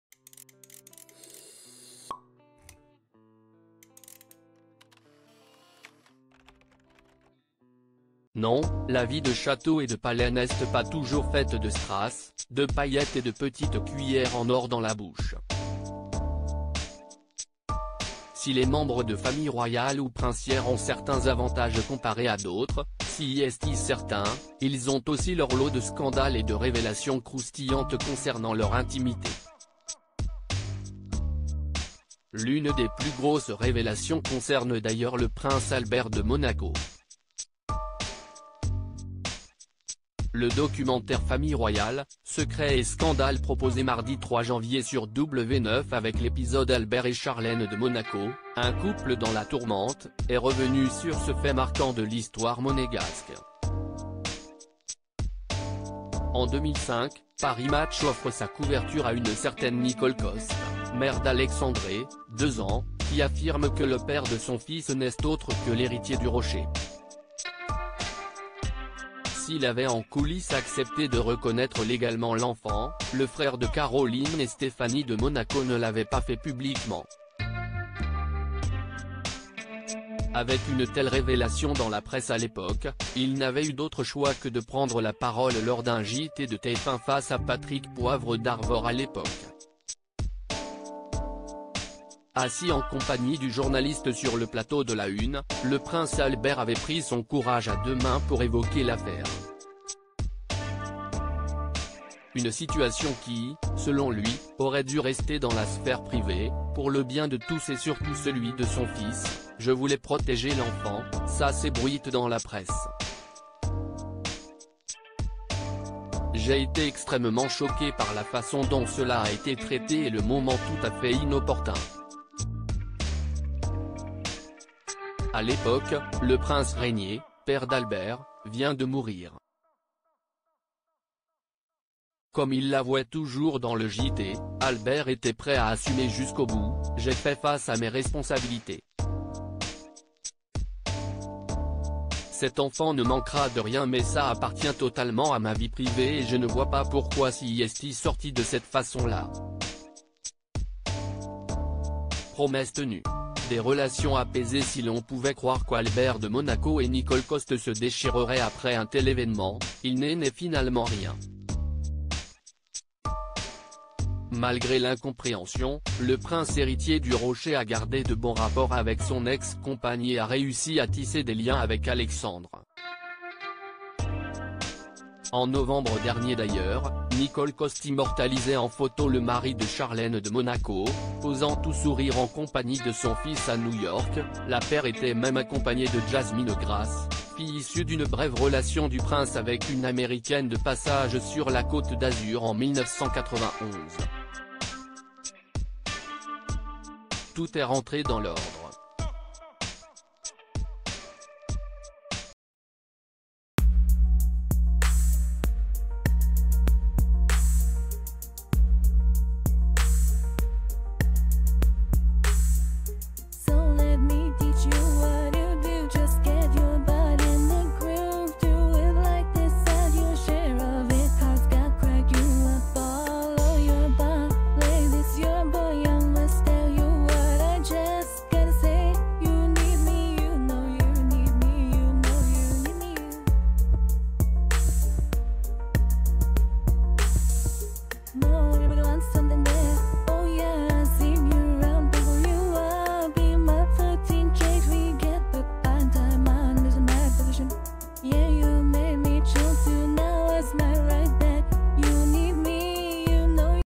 Albert de Monaco et Alexandre, son fils d'abord caché : le prince "extrêmement choqué" par la révéla
Albert de Monaco et Alexandre, son fils d'abord caché : le prince "extrêmement choqué" par la révélation